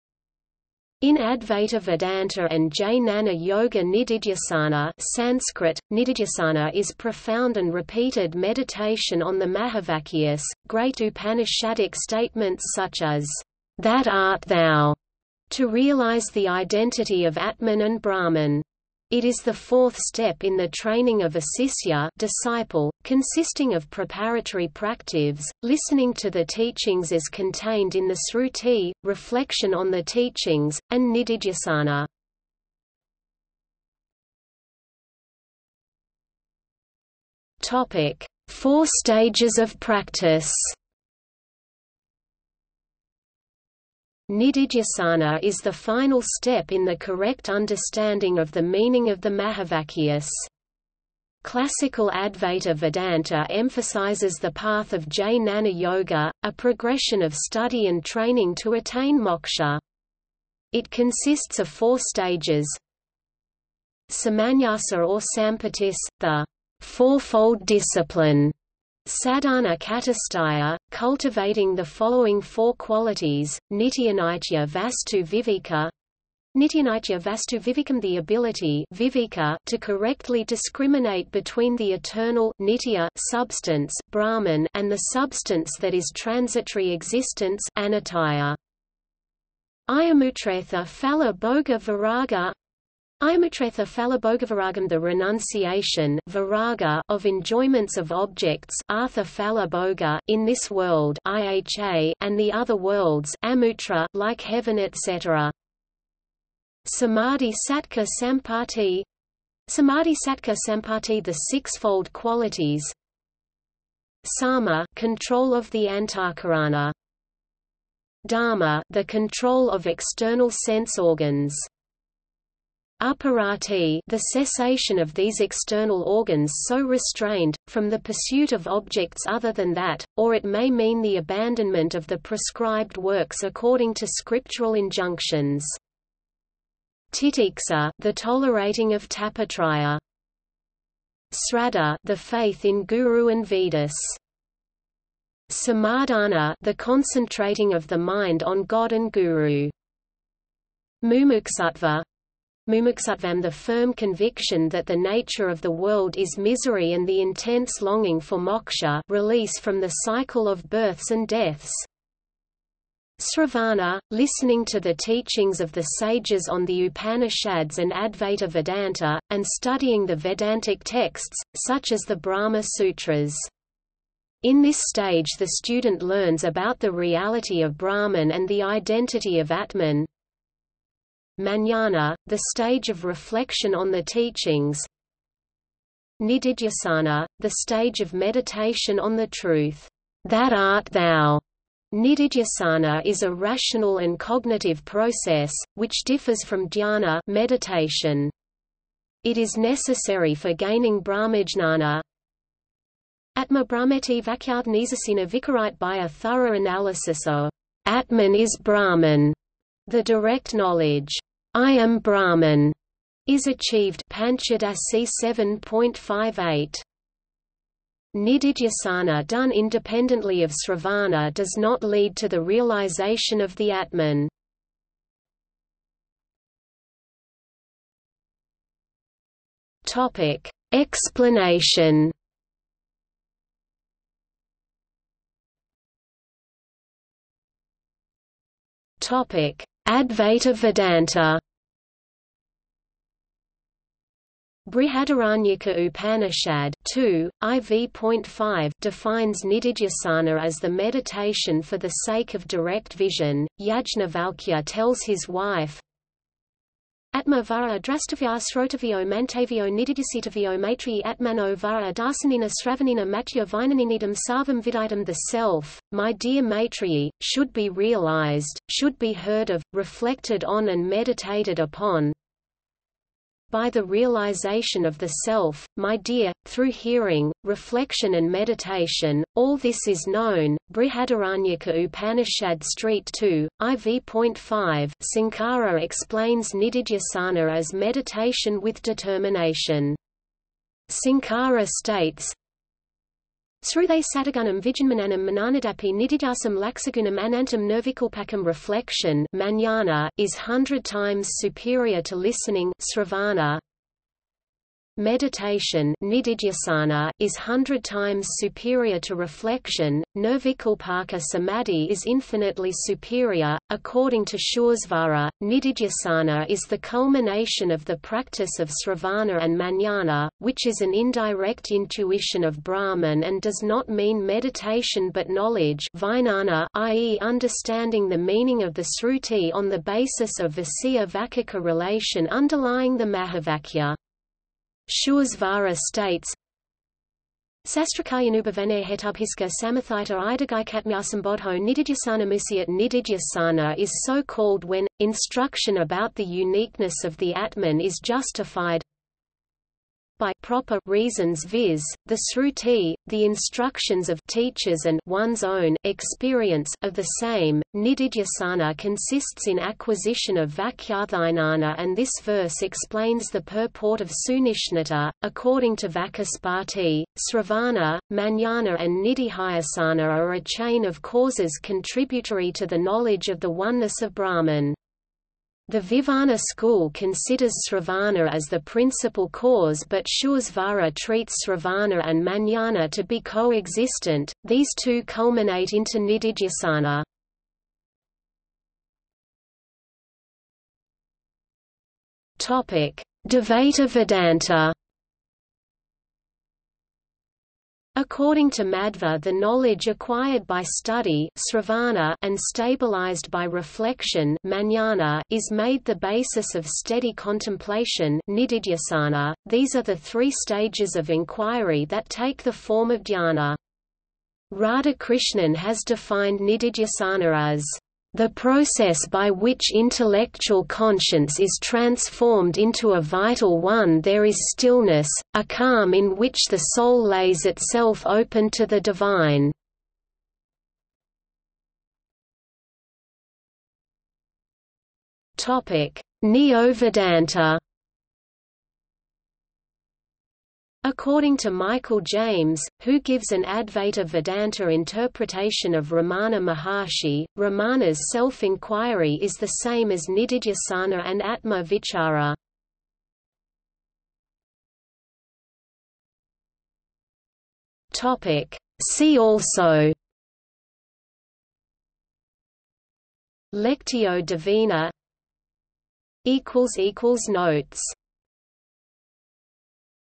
Nididhyasana | Wikipedia audio article
00:00:34 1 Four stages of practice
00:03:46 2 Explanation
00:03:55 2.1 Advaita Vedanta
00:06:29 2.2 Dvaita Vedanta
00:07:12 2.3 Neo-Vedanta
00:07:34 3 See also

- Socrates

SUMMARY
In Advaita Vedanta and Jnana Yoga Nididhyasana (Sanskrit: निदिध्यासन) is profound and repeated meditation on the mahavakyas, great Upanishadic statements such as "That art Thou", to realize the identity of Atman and Brahman. It is the fourth step in the training of a sisya (disciple), consisting of preparatory practives, listening to the teachings as contained in the sruti, reflection on the teachings, and nididhyasana.